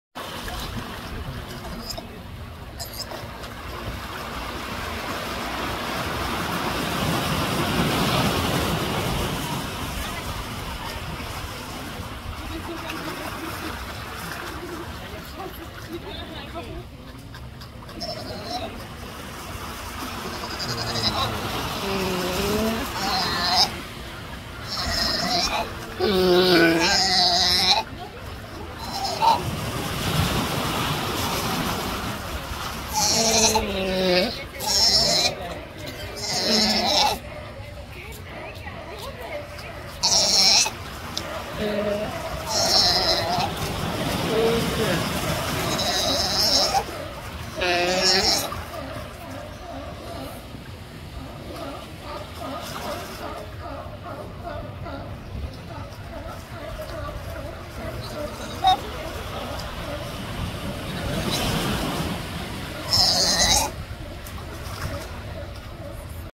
newborn sea lion gets some rough handling by its momma
This sea lion mom just can't seem to find the right spot to put her newborn pup.